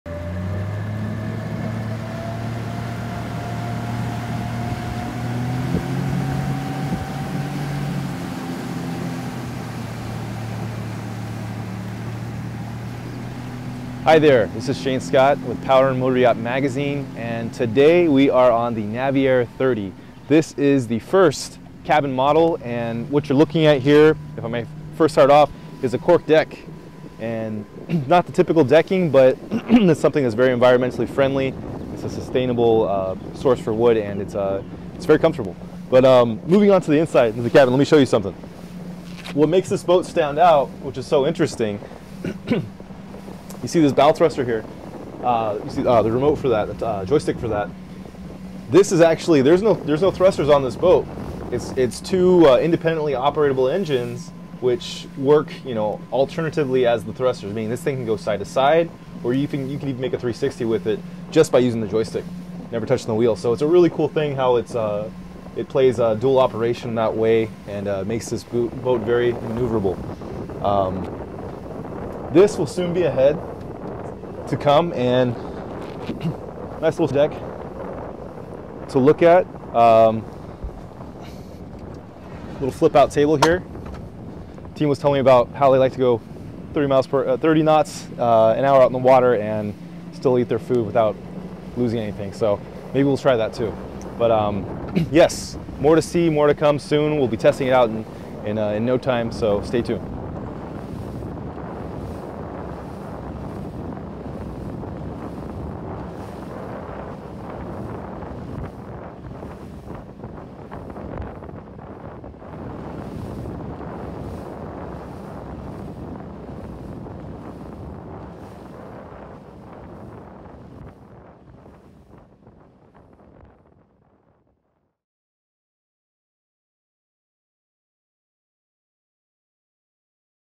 Navier 30 Hydrofoiling Electric Boat
Testing the Electric-Powered, Hydrofoiling Navier 30. Power & Motoryacht test drives the world’s longest-range electric boat on San Francisco Bay.